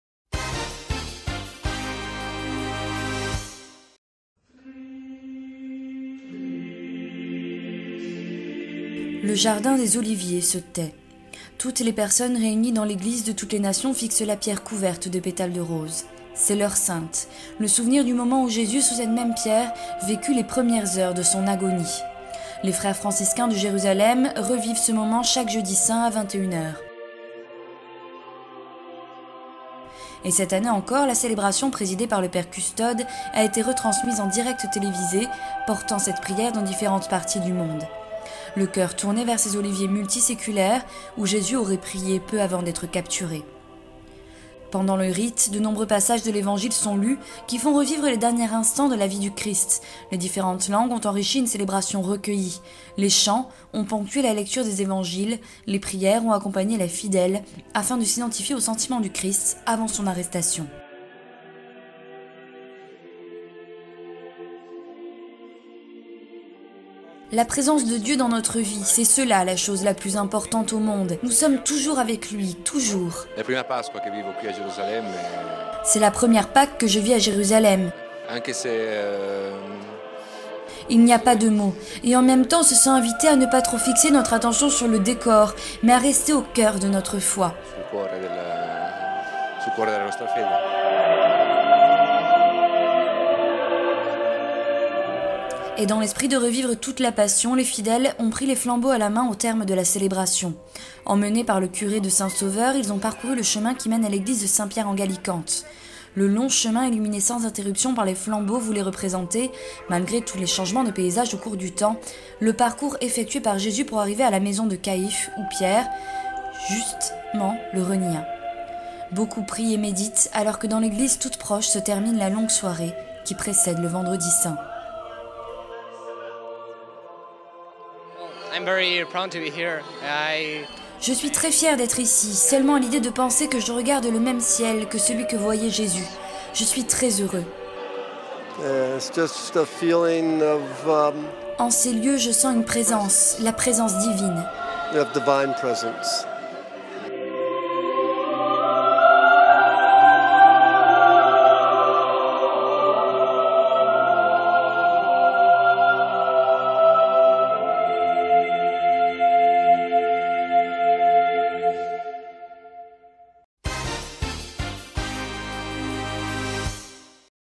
La Passion du Jeudi Saint
La célébration de l'Heure Sainte à Getsémani en direct TV. La procession jusqu'à l'église de Saint-Pierre-en-Gallicante pour se rappeler l'arrestation de Jésus. Le Jeudi Saint est marqué par les célébrations franciscains qui se prolongent jusque tard dans la soirée.